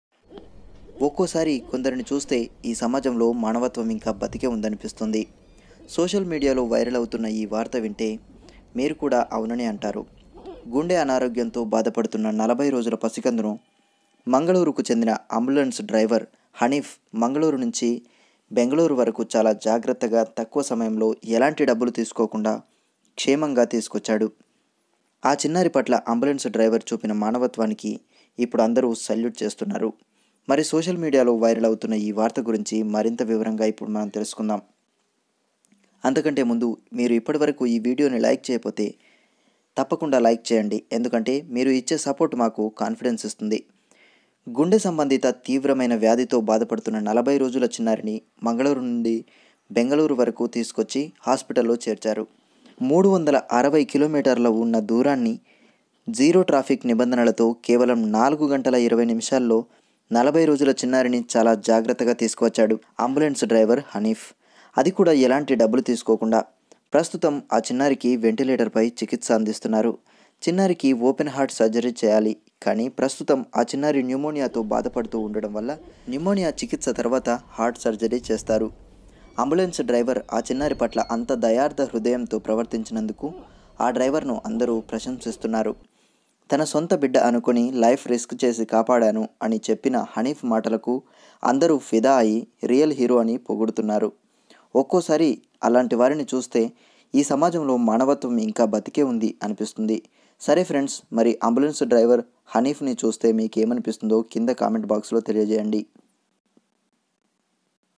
mangalore to banglore || 40 days baby || ambulance driver || mohammad hanif ||heart transplant
40 రోజుల చిన్నారిని 4 గంటల్లో 400 కిలోమీటర్లు తీసుకువచ్చి కాపాడిన అంబులెన్స్ డ్రైవర్ మహమ్మద్ హనీఫ్